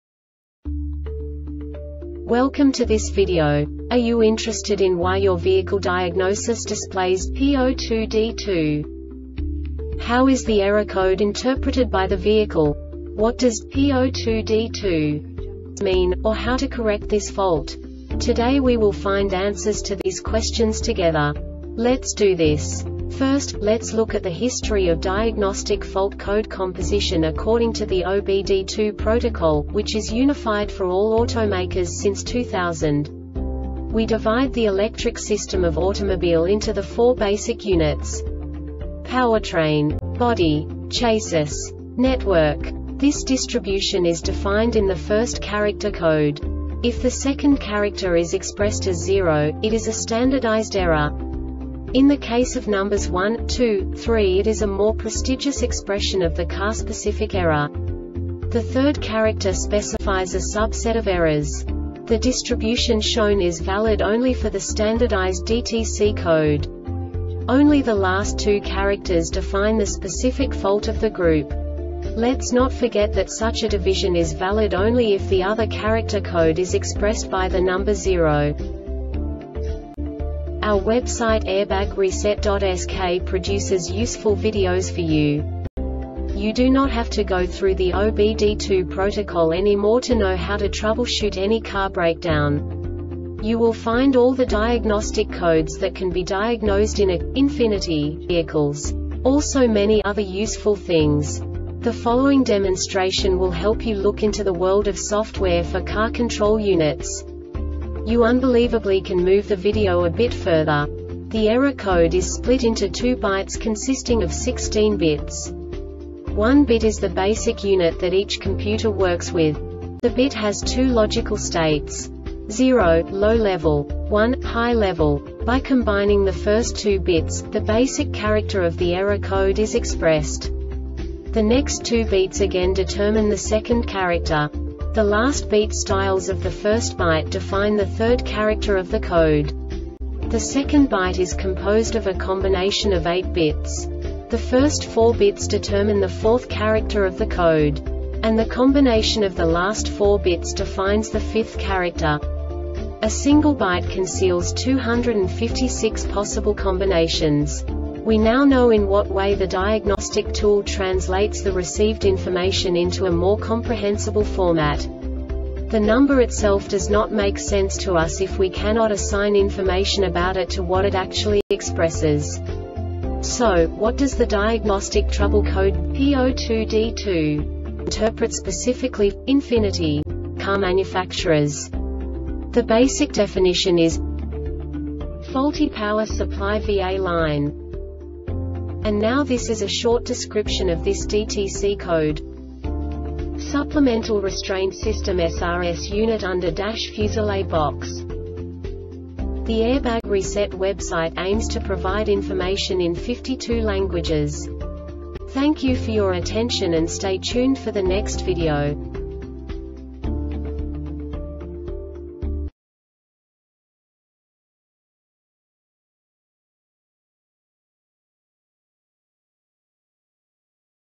DTC Ford P02D2 Short Explanation
Contents:
0:21 Basic DTC analysis according to OBD2 protocol standard.
1:48 Insight into programming
2:58 A diagnostically understandable description of the Diagnostic Trouble Code
3:05 Basic error definition

Make:
Ford
Code:
P02D2
Definition:
Faulty Power Supply (VA Line)
Cause:
Supplemental restraint system (SRS) unitUnder-dash fuse or relay box